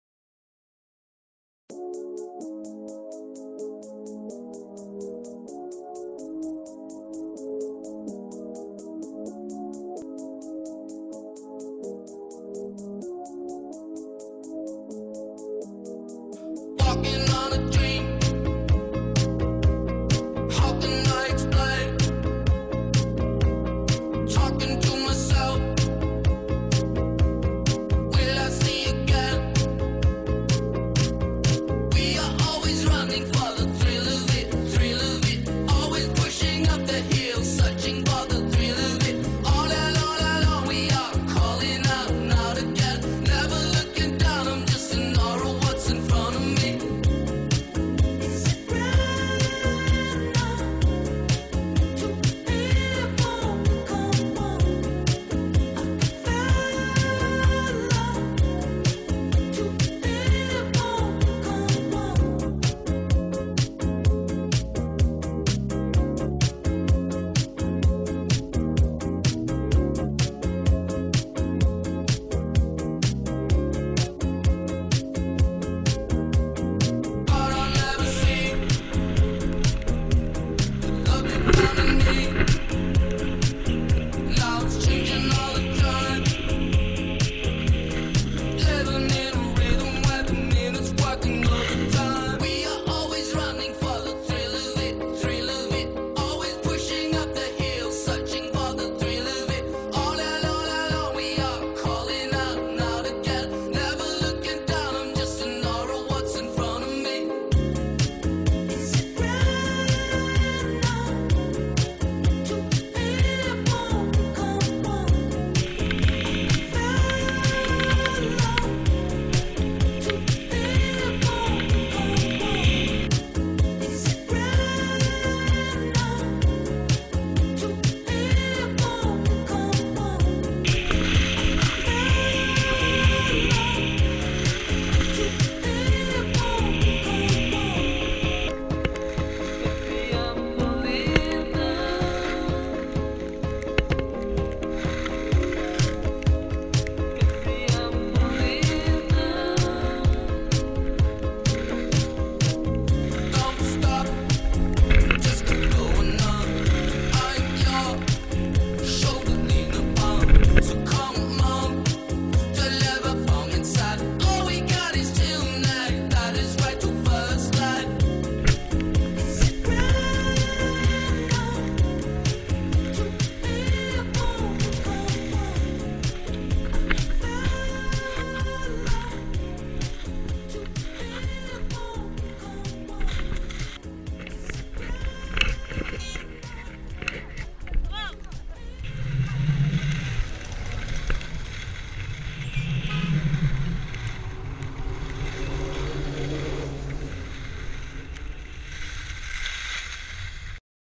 Selfies and Fresh Powder - Smuggs Day 3
Tori, Trey, and Dad hit the mountains after a fresh snow...with a selfie stick